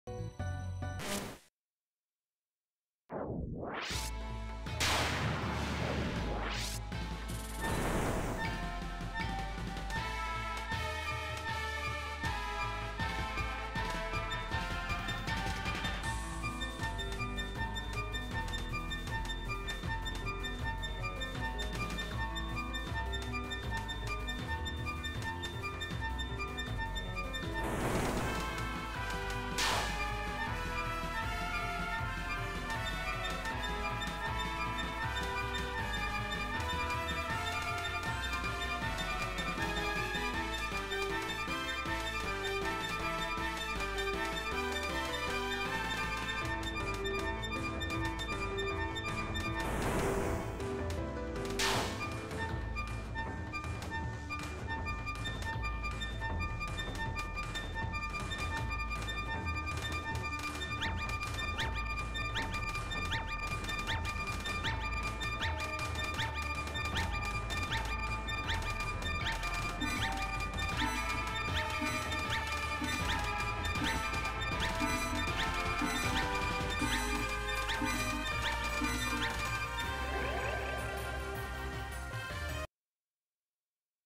Blue Lady Room Capture
So this wasn't as bad as I thought it be.  Pretty much comes down to learning the final part, otherwise you're pretty much out of luck.  I definitely got a fluke in the ending but it wouldn't be too hard to get another clear.